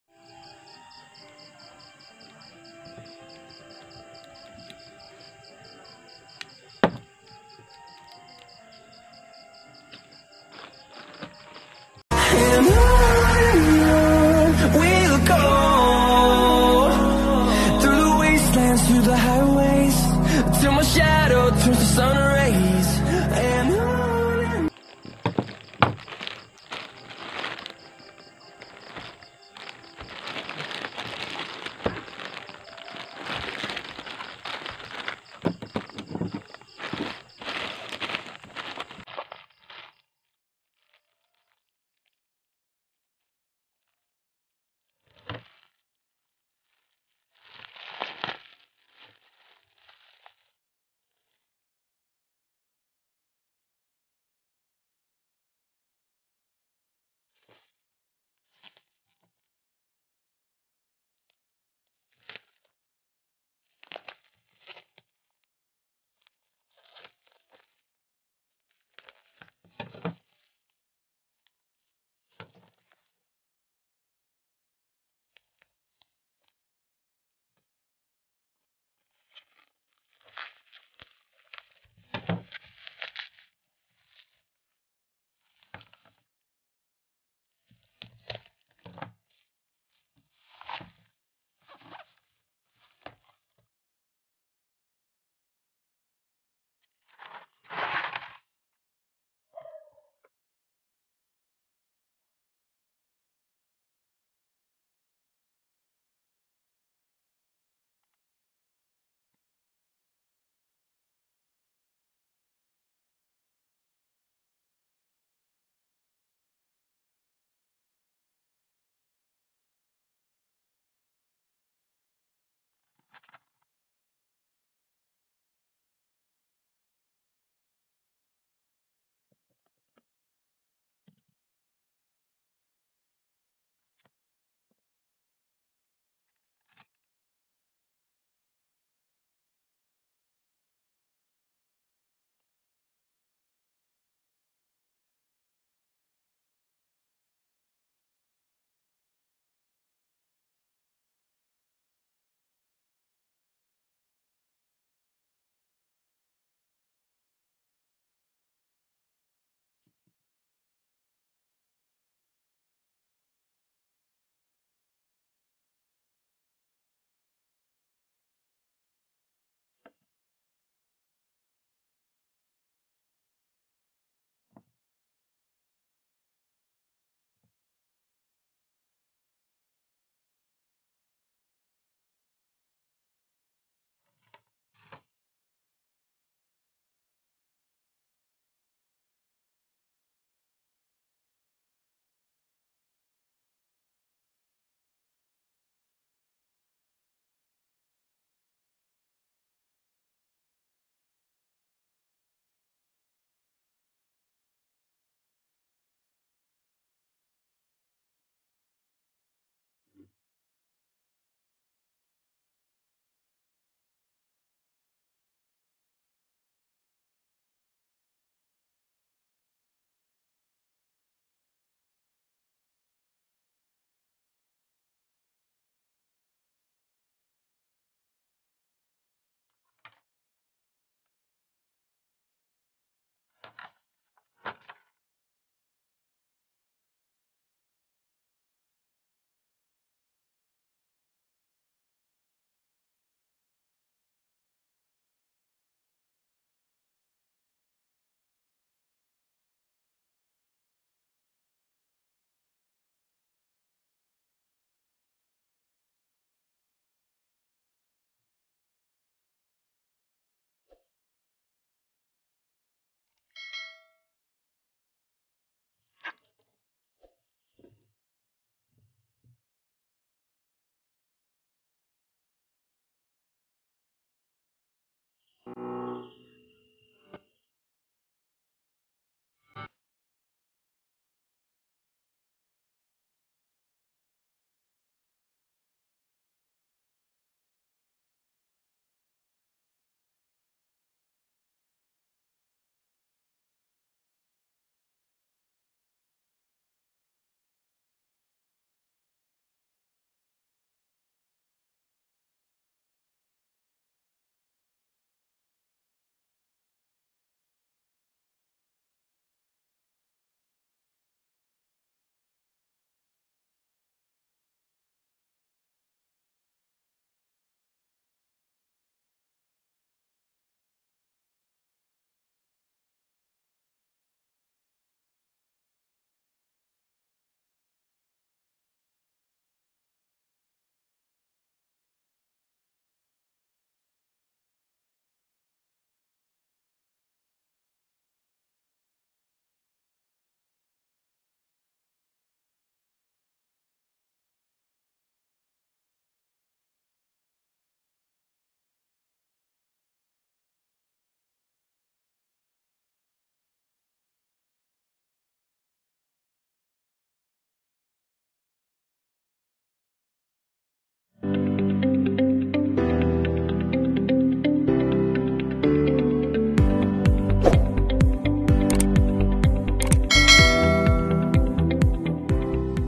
Unboxing and assembling of Video-making Kit AY-49
Unboxing and assembling of Video-making Kiit AY-49 Ramil Tombado Vlogs wants to share you how to assemble video-making kit for vlogging.